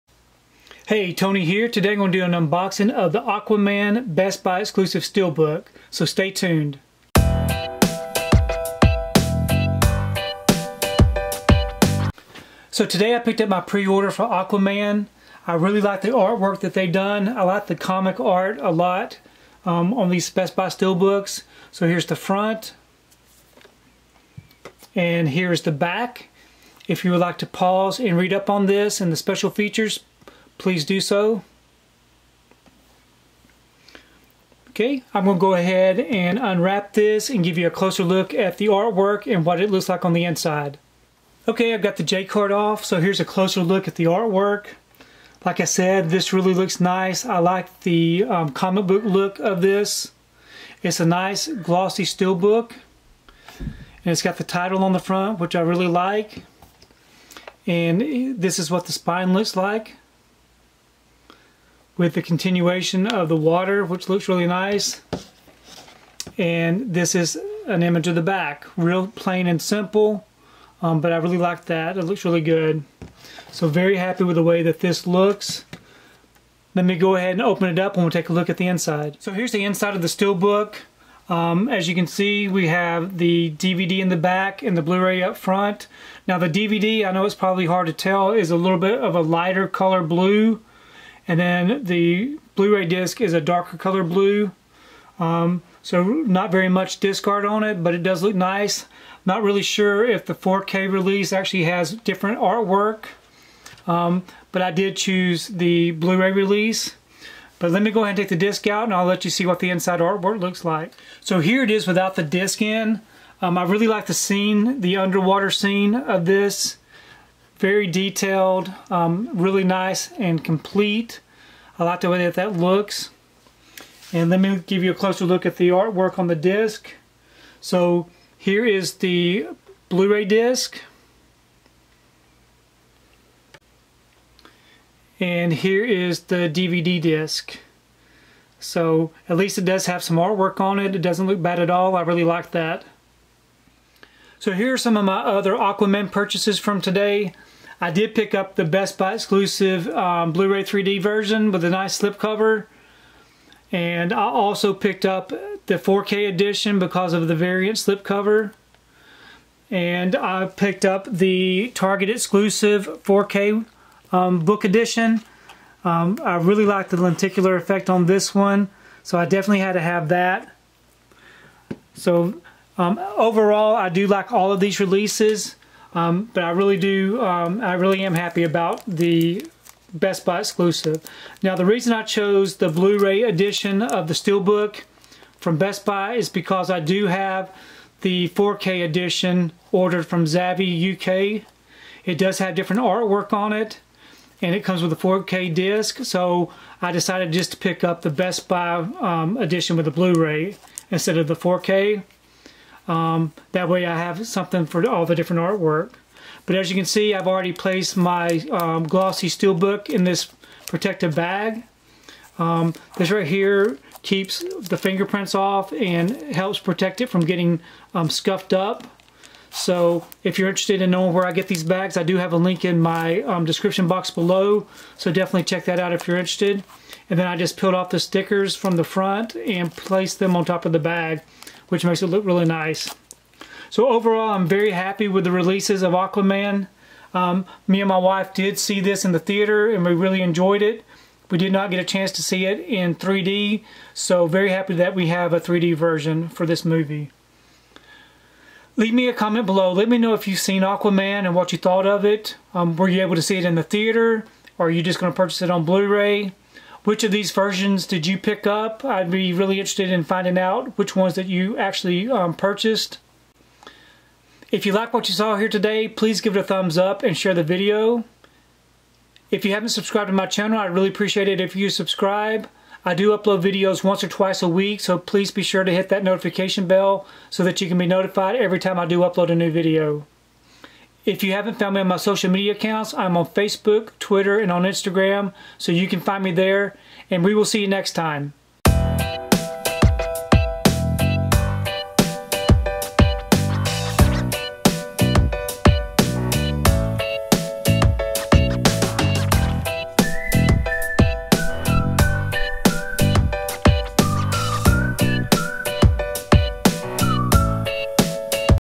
Aquaman Best Buy Exclusive Collectible Blu-ray SteelBook Unboxing
Check out my unboxing of Aquaman Best Buy Exclusive Collectible Blu-ray Steelbook!

► Purchase the Blu-ray